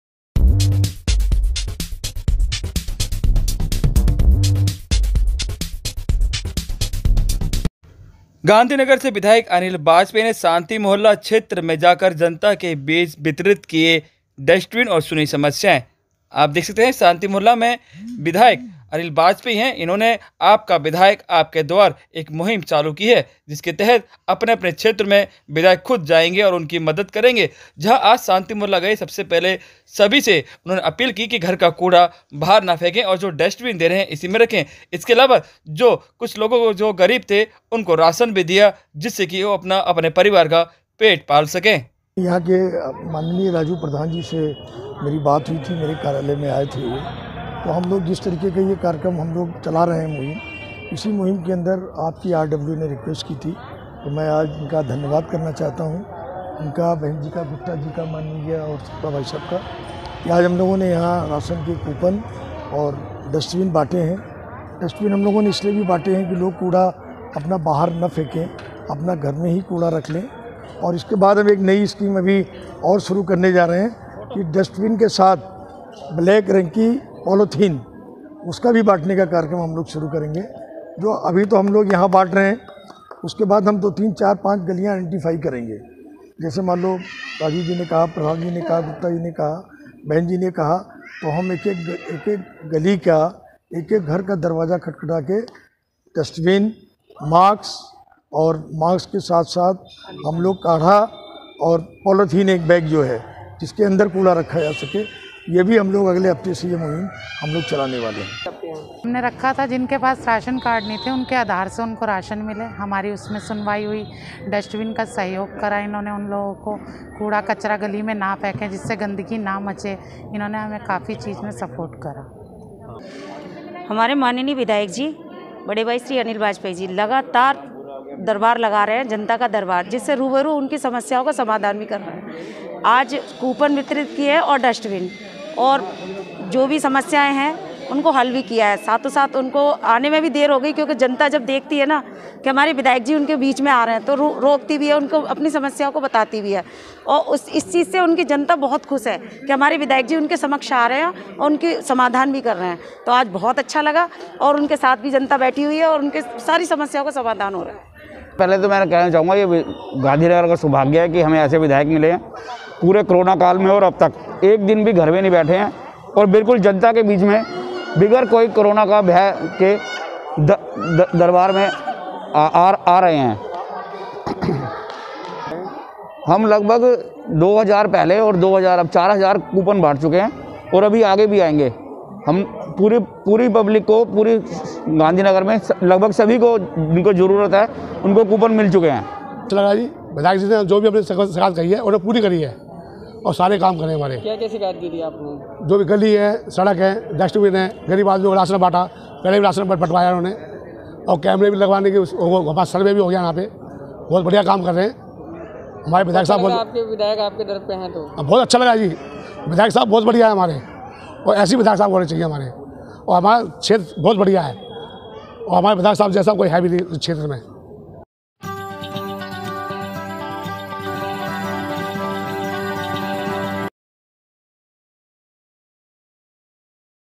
Gandhi Nagar MLA अनिल बाजपाई जी ने शांति मोहल्ला में जनता के बीच जाकर डस्टबिन वितरित किए
पूर्वी दिल्ली गांधीनगर विधानसभा क्षेत्र से विधायक अनिल बाजपेई ने आपका विधायक आपके द्वार के तहत शांति मोहल्ला जनता के बीच जाकर दर्शन दिए और जरूरतमंद लोगों को राशन के लिए कूपन दिए जनता ने खुशी जताई